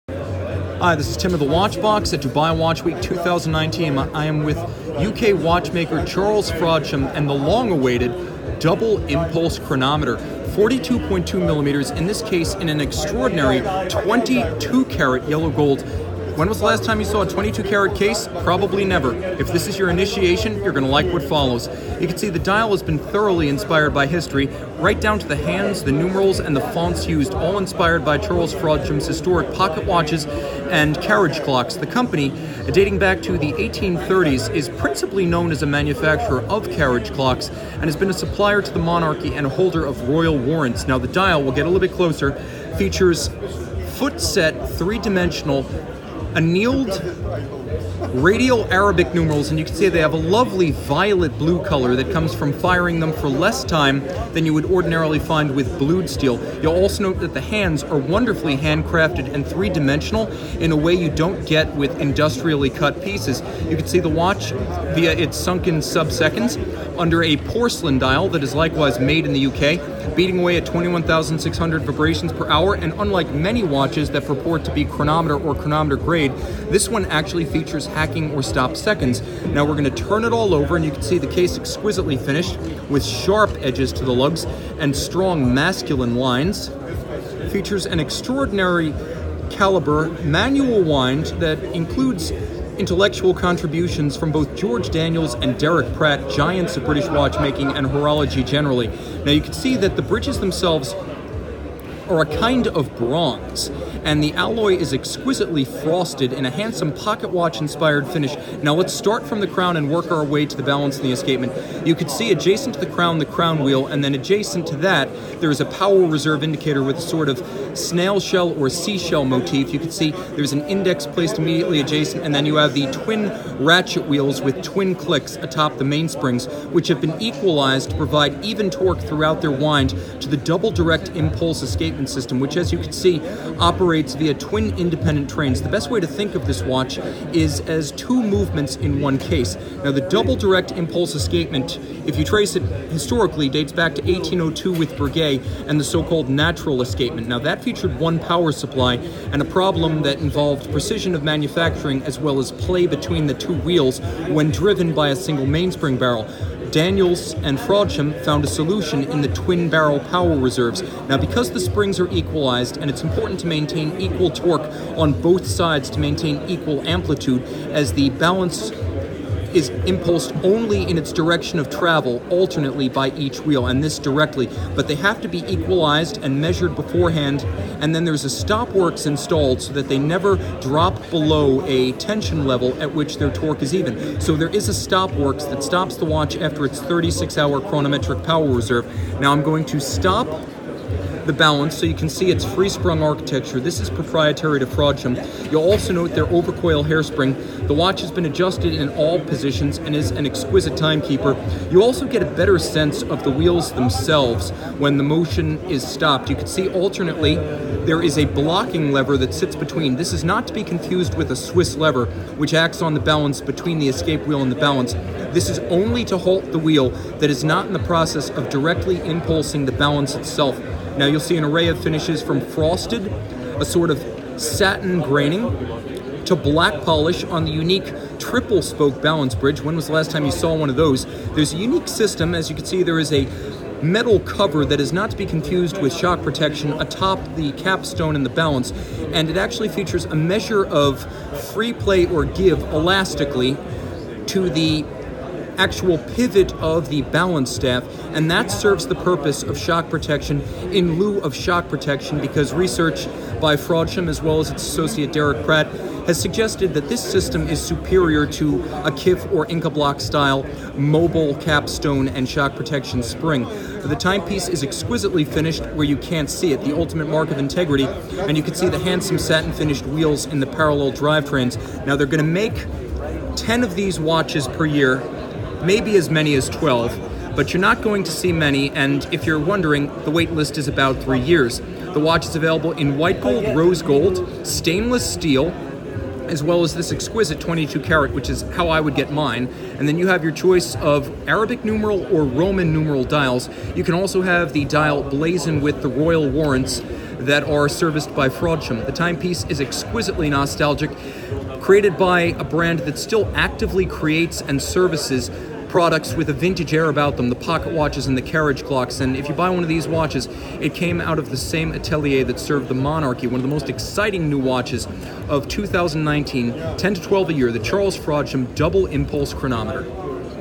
2019 Charles Frodsham Double Impulse Chronometer: Dubai Watch Week 2019
The 2019 Charles Frodsham Double Impulse Chronometer has been described as the most exciting new independent brand watch of this model year. With a 42.2mm 22-karat gold case, a porcelain dial, and a double direct-impulse movement that incorporates the input of George Daniels and Derek Pratt, the UK-manufactured Charles Frodsham boasts impressive pedigree.

Nearly 20 years of development informed the final design of this Charles Frodsham watch. The earliest innovation was George Daniels' double-direct impulse escapement driven by two independent barrels and going trains. The system is a descendant of Abraham Louis Breguet's "natural escapement" of 1802. Since both wheels impulse the balance exclusively in its direction of travel, the frictional losses of a conventional Swiss lever escapement are minimized. British watchmaker and Daniels contemporary, Derek Pratt, offered specific guidance concerning the escapements and implementation of the Daniels concept.

A dial of British-built porcelain features a composition similar to ceramic and a set of fired steel Arabic numerals mounted by protruding "feet." Both Roman numerals and Arabic numerals are available with "radial" setting. At center, the equal-length hands are constructed and annealed using manual means, and the violet tones are heat-tempered to match the color of the numerals. Charles Frodsham specifically chose "London hallmarked" 22-karat yellow gold for the case on the basis of this material's exceptional tone.

Inside the Double Impulse Chronometer, a manual-wind movement with a 36-hour power reserve keeps the time. Peripheral refinements include a full balance bridge, a freesprung balance, a hand-made Breguet overcool, and a caseback power reserve indicator. The 21,600 vph double-direct impulse escapement operates unlubricated for greater efficiency, and a stopworks halts the mechanism once the mainsprings reach the limit of their chronometric power reserves.

For complete details, watch the full Charles Frodsham Double Impulse Chronometer review by Tim Mosso!